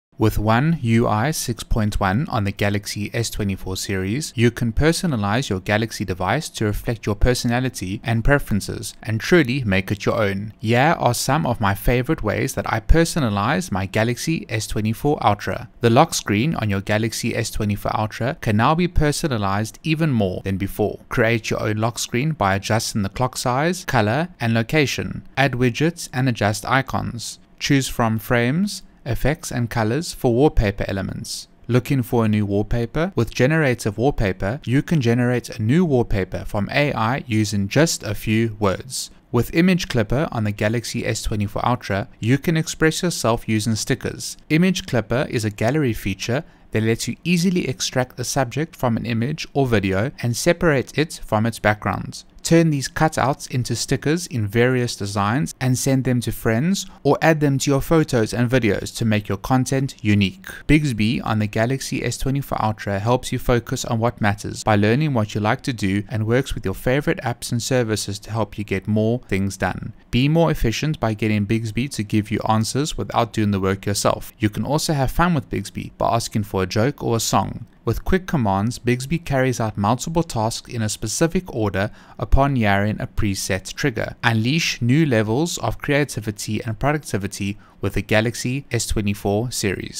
Samsung Galaxy S24 Ultra One U.I 6.1 Personalisation
With One U.I 6.1 on the Galaxy S24 Series, you can personalise your Galaxy device to reflect your personality and preferences, and truly make it your own! Here are some of my favourite ways that I personalised my Galaxy S24 Ultra.

Personalisation: The lock screen on your Galaxy S24 Ultra can now be personalized even more than before. Create your own lock screen by adjusting the clock size, colour and location. Add widgets and adjust icons. Choose from frames, effects and colours for wallpaper elements. Looking for a new wallpaper? With Generative Wallpaper, you can generate a new wallpaper from AI using just a few words!

Image Clipper: With Image Clipper on the Galaxy S24 Ultra, you can express yourself using stickers. Image Clipper is a gallery feature that lets you easily extract a subject from an image or video and separate it from its background. Turn these cut outs into stickers in various designs, and send them to friends or add them to your photos and videos to make your content unique.

Bixby: Bixby on the Galaxy S24 Ultra helps you focus on what matters by learning what you like to do and works with your favourite apps and services to help you get more things done. Be more efficient by getting Bixby to give you answers without doing the work yourself. You can also have fun with Bixby, by asking for a joke or a song! Need help getting more work done? With Quick Commands, Bixby carries out multiple tasks in a specific order upon hearing a preset trigger and you can even create your own Quick Commands. Unleash new levels of creativity and productivity with the Galaxy S24 Series!